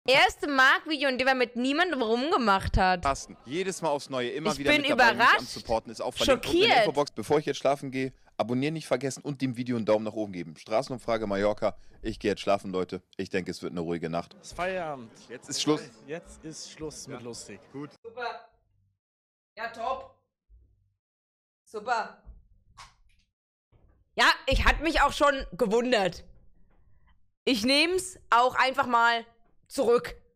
"Marc hat diesmal mit keiner rumgemac-..." | Reved Clips
@Reved reagiert auf ein Video von @thatsmemarc